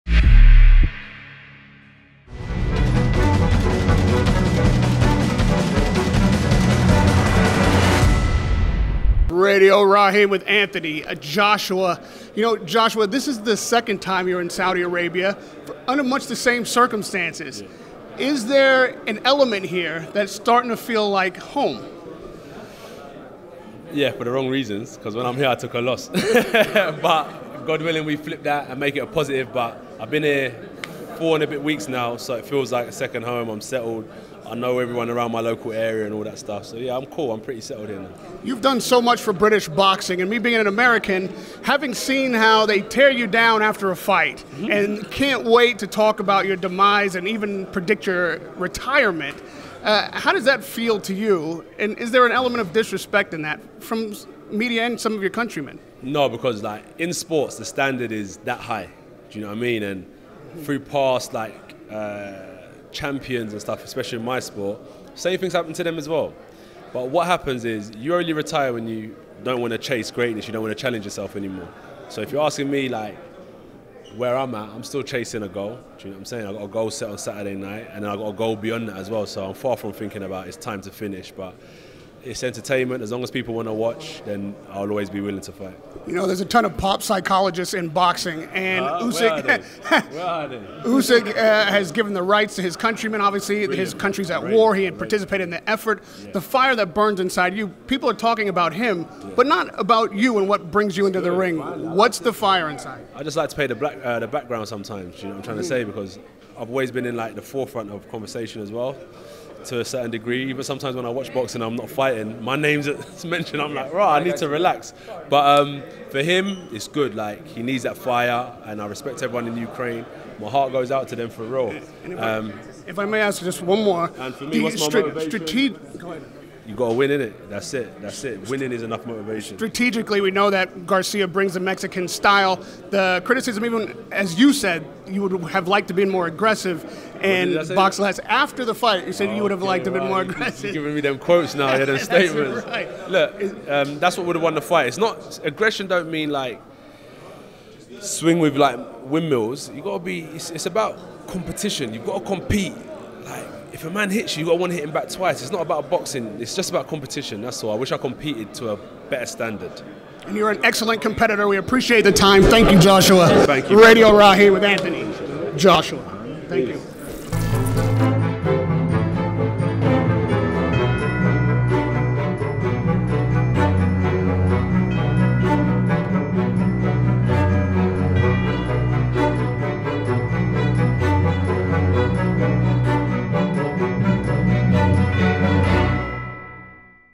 ANTHONY JOSHUA: Aggression Would have Beaten Usyk!
Interviewed by Radio Rahim in Jeddah | Usyk vs Joshua 2